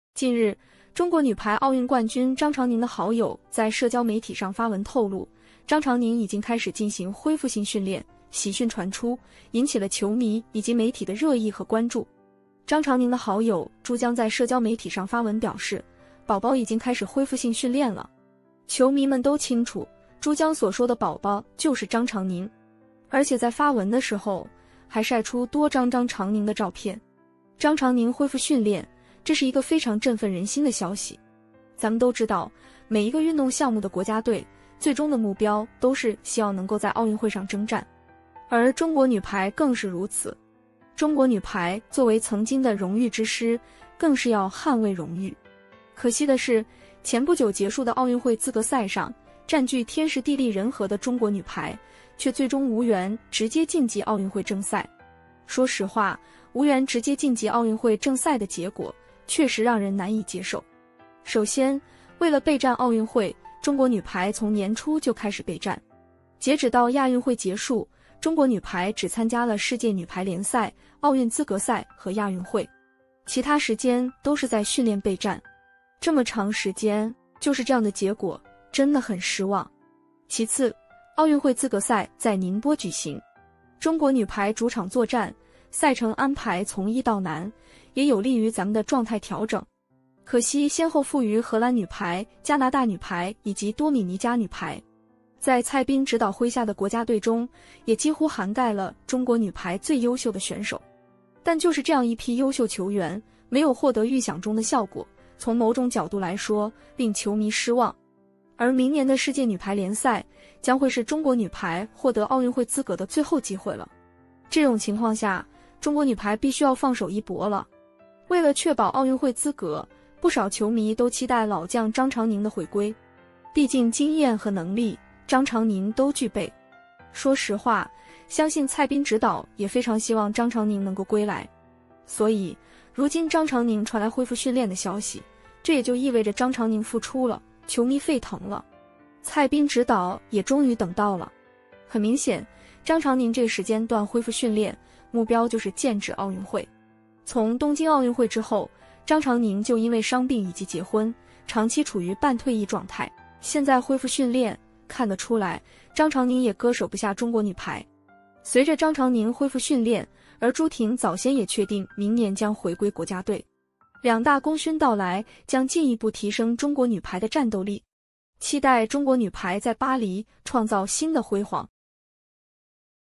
恭喜中国女排，张常宁归队重大突破，知情人曝内情，朱婷，张常宁合体倒计时
张常宁归队重大突破，知情人曝内情，朱袁张合体倒计时